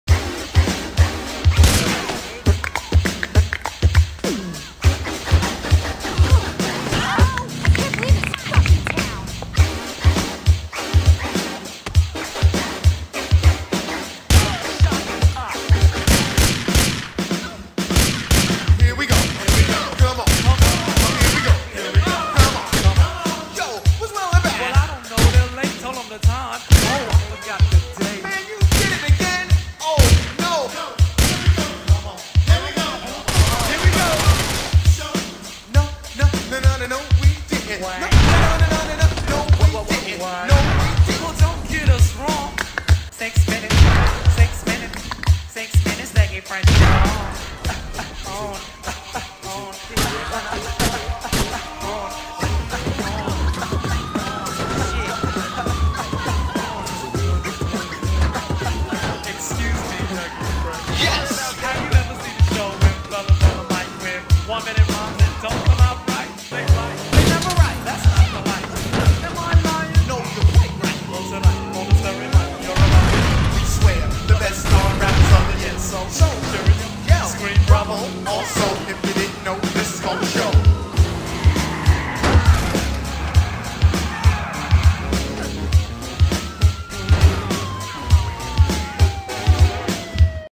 O MY GOD
Kicking, shooting and exploding people off of a high ledge. Then there is some sidewalk bowling from interesting angles, with an exploding gas station in the mix.

Thinning the Digital Herd - Kick 'em in the Face Style!